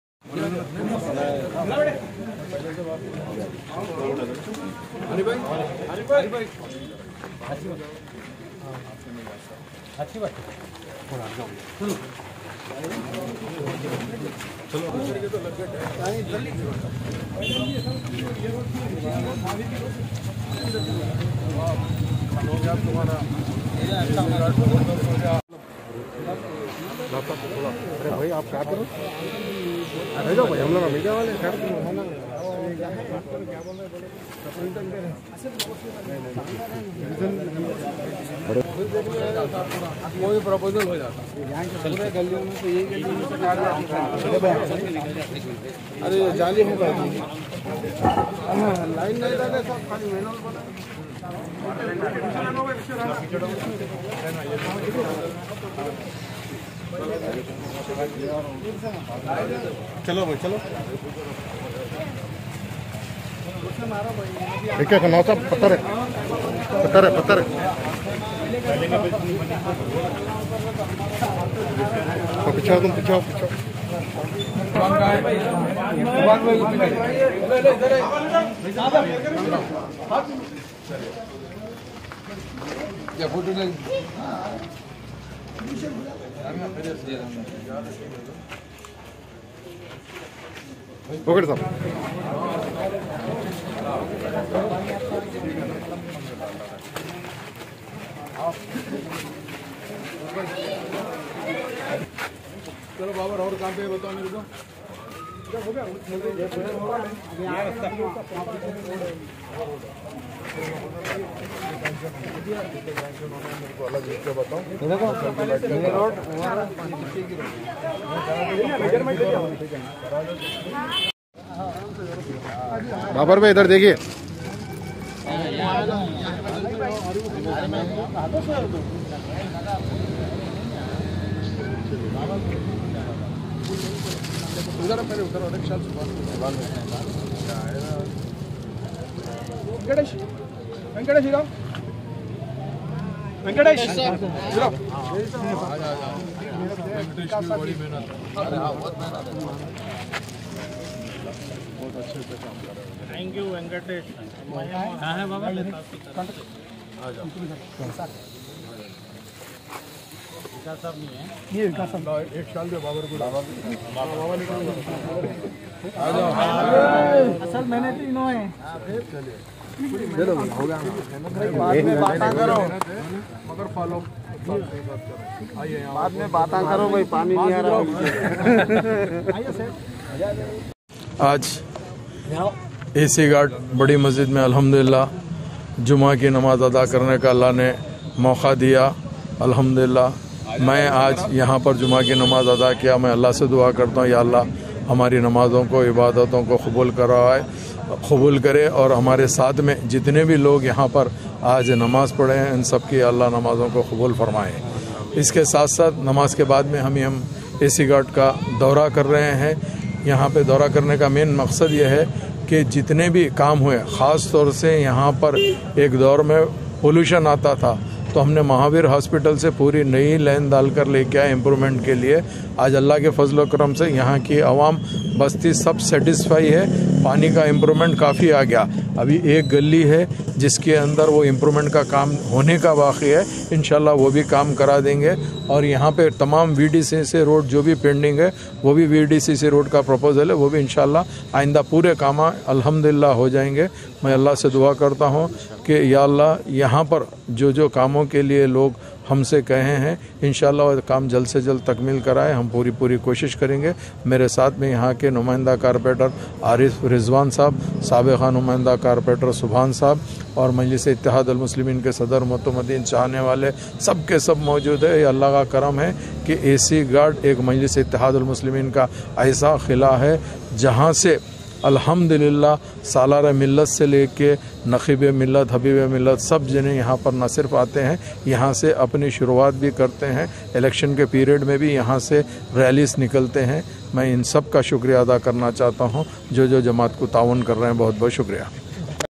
MIM MLA Jaffar Hussain Meraj Ne Kiya AC GUARD Ka Daura
After Offering Namaz E Juma in Badi Masjid, I Inspected A.C Guard and Surrounding Areas and also met people to listen and solve their Civil Grieviences along with Arif Rizwan (Redhills Numainda Corporator), Subhan ( Ex Numainda Corporator, Redhills ),and AIMIM Primary Unit Members of Redhills.